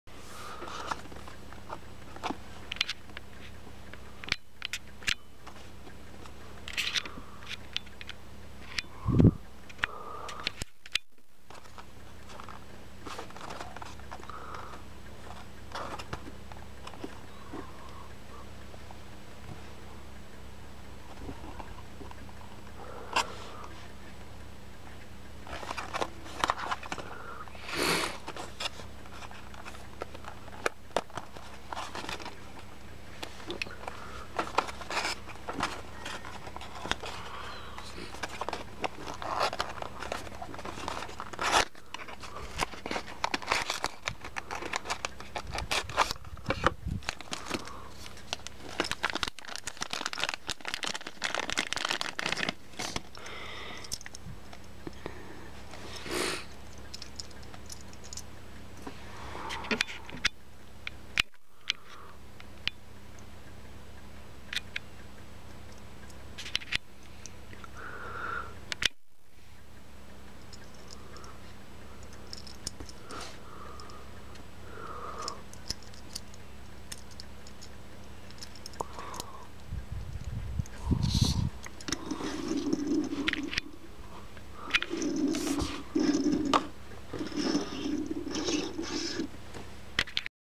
ドリームトミカ マリオカート7 ヨッシー 開封
ドリームトミカ マリオカート7 ヨッシーを開封します。
かっこいいと言うよりも可愛いです。